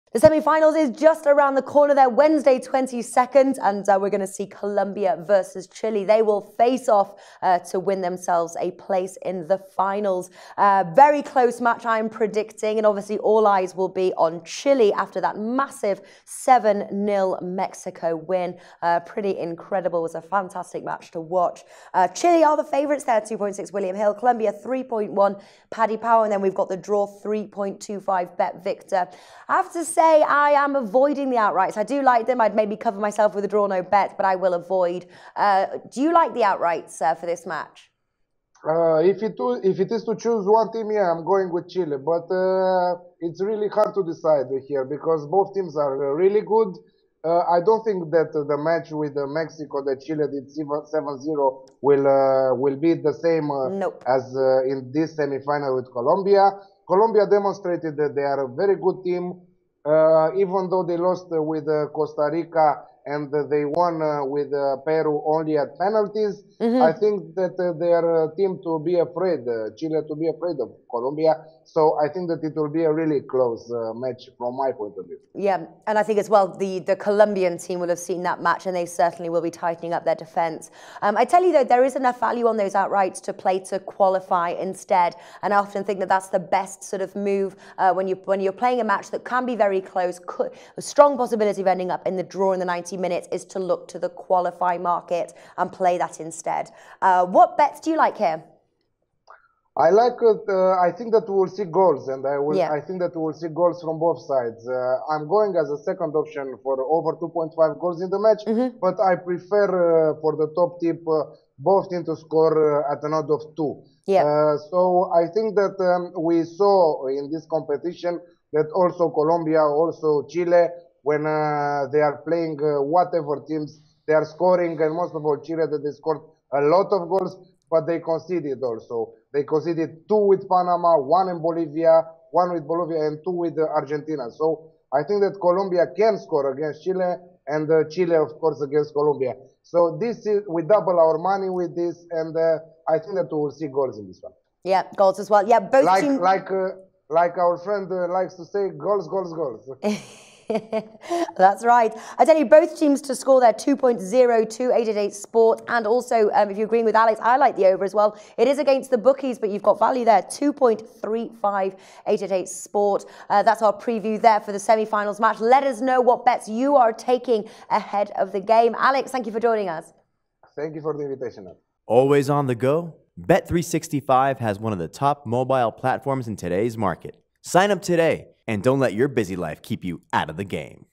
Colombia vs Chile | COPA AMERICA 2016 | Semi Finals Match Predictions
Natalie Rydstrom @NatalieRydstrom and Alex from MBitCasino.com come together to discuss the Wednesday 22nd June Copa America 2016 semi finals match Colombia vs Chile match and the best plays they like for the game.